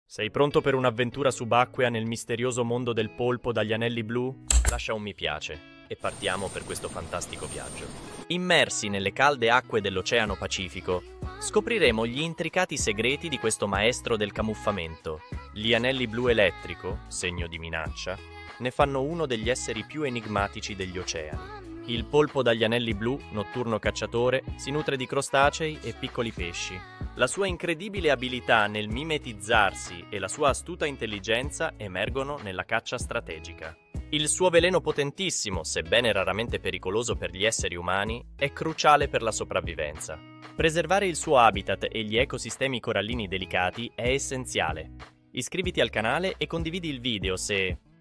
GUARDIANI delle BARRIERA CORALLINA 🐙🌺: Il POLPO dagli ANELLI BLU/ 5 CURIOSITÀ
Benvenuti a bordo dell'esplorazione subacquea del secolo! Siete pronti per scoprire i segreti del polpo dagli anelli blu? 🌊🐙
Unisciti a noi nel viaggio attraverso le acque misteriose dell'Oceano Pacifico, alla ricerca di uno degli esseri più affascinanti degli oceani!
Scopriremo gli anelli blu elettrico, il camuffamento incredibile, e le incredibili abilità di caccia di questa creatura notturna. Ma c'è di più...